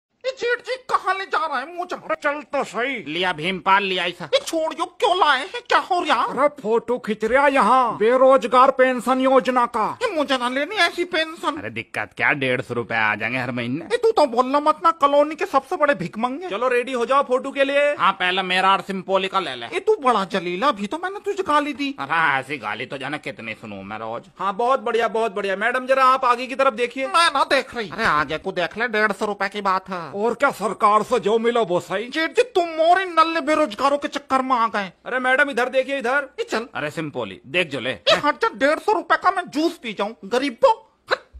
cimpoli bhabhi gayi jethji ke sath photo khichne berojgari ke liye to my YouTube channel
my name Rohit shivhare
my dream is Comedy Moovie
 I am The comedy scenes from dabra to Gwalior